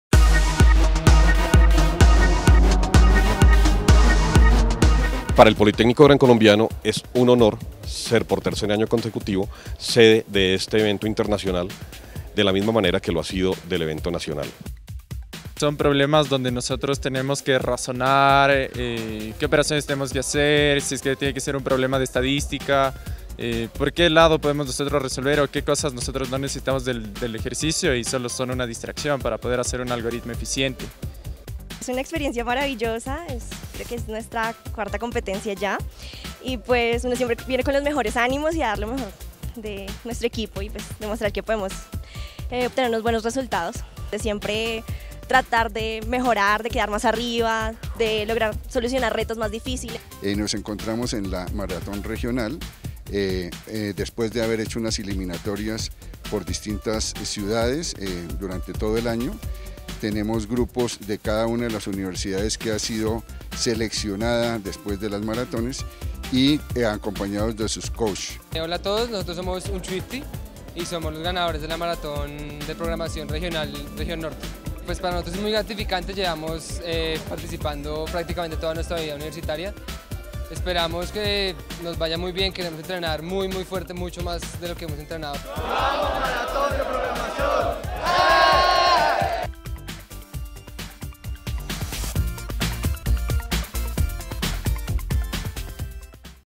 Maratón Regional de Programación 2017
Así vivimos la Maratón Regional de Programación que con orgullo se volvió a celebrar en el Politécnico Grancolombianio